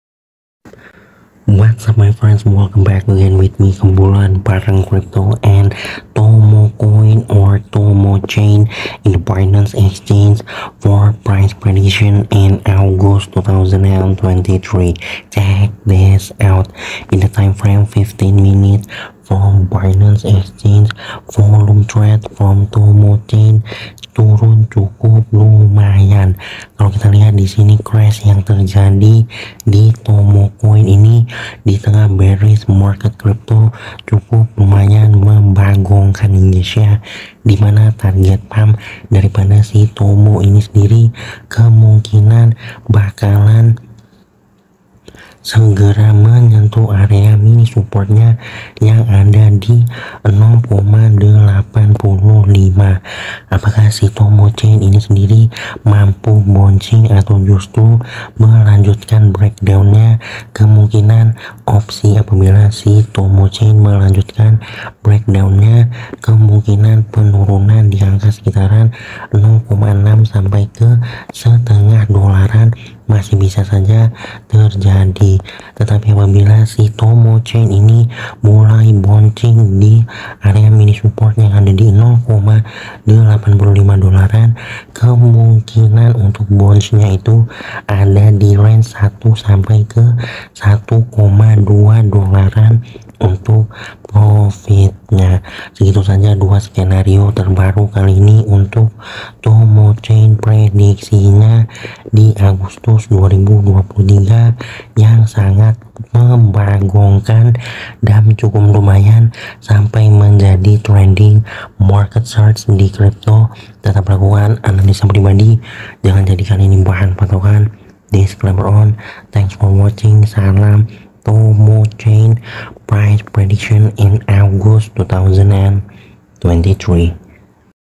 TOMO COIN IS GOING TO CRASH LOWER IN AUGUST 2023❗ TOMOCHAIN PRICE DROP TARGET❗ TOMO EMERGENCY UPDATE
tomo coin is going to crash lower in august 2023 tomochain crypto price drop target in august 2023